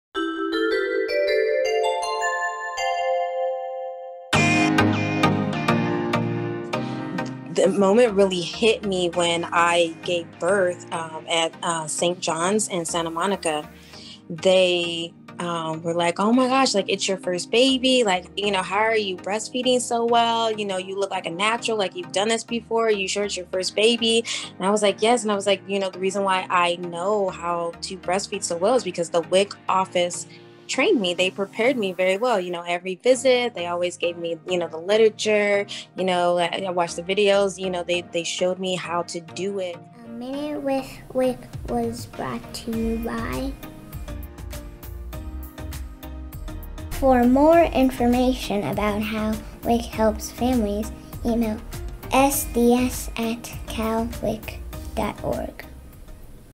Minute with WIC | WIC is So Much More Than Food Benefits
Moms appreciate the breast-feeding support they receive from WIC. Our mission is to lead California communities to nourish, educate, support, and empower families in building a healthy future.